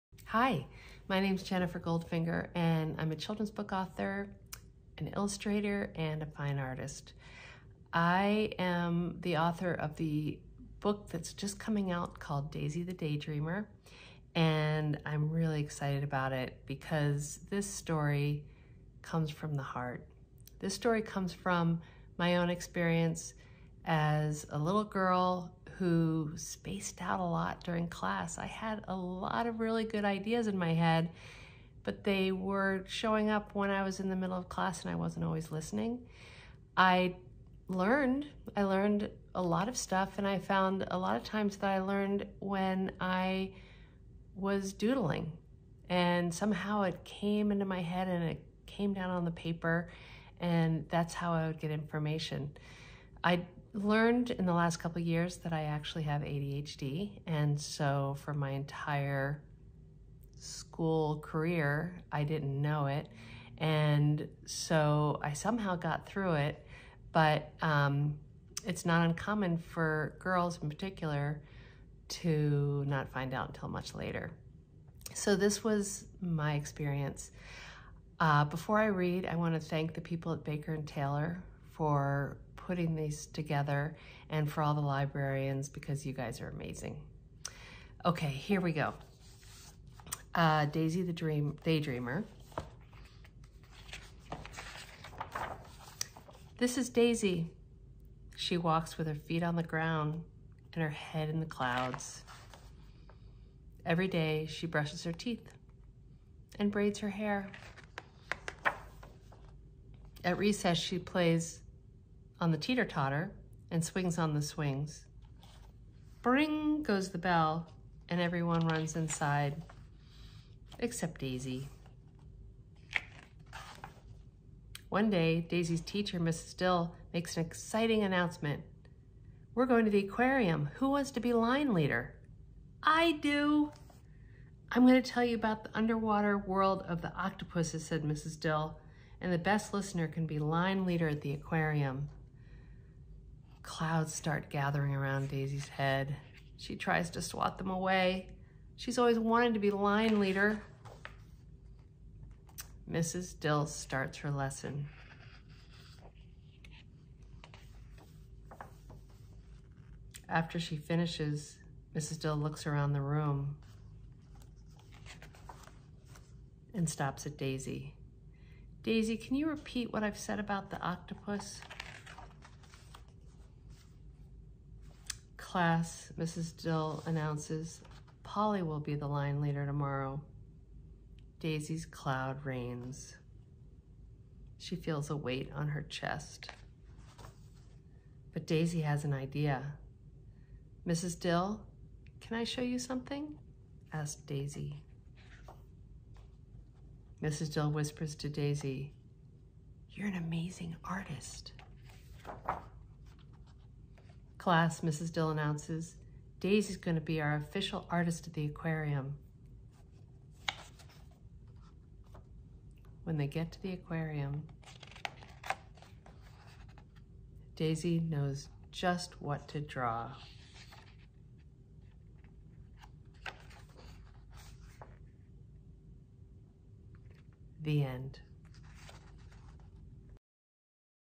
First Chapter Friday: Jennifer P. Goldfinger
Presenting a new Baker & Taylor First Chapter Friday! This week we're excited to share a video from Jennifer P. Goldfinger introducing and reading from her new book Daisy the Daydreamer.